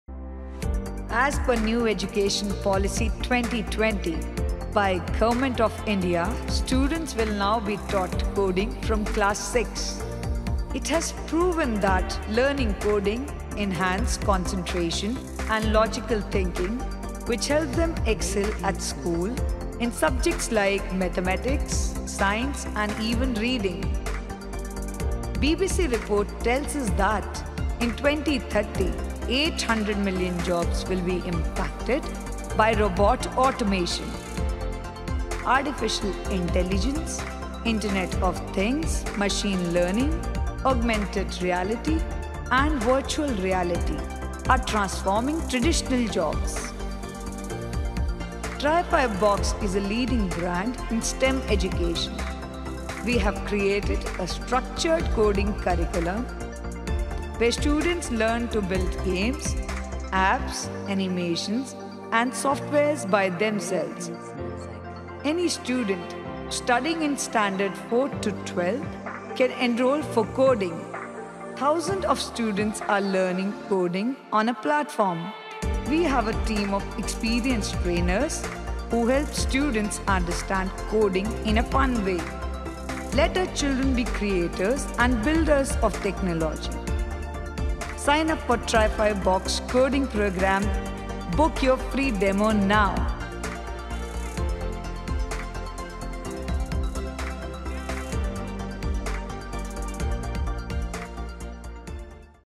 Tryfibox Coding Program | NEP 2020
As per the National Education Policy by government of India, coding will be taught from class 6th onward. Also Artificial Intelligence will be taught from class 9th onward.

In today's Life, technology is playing a very important role in every aspect of life. Technology made everything so easy that every bit of information is just one click away. This is possible because of the several applications generally called as software or apps.

Coding (or Programming) is the process to develop the programs and software. So, to develop such applications and to be a developer rather than a simple user, students need to learn how to code.

Coding is used for Mobile App &Software development, Game development, Artificial Intelligence, Machine Learning and Web Development etc.

As per New Education Policy 2020, we are providing a variety of courses for skill development of students and making them future ready with 21st century skills.

Get latest updates on STEM learning, Robotics, DIY Activities and much more…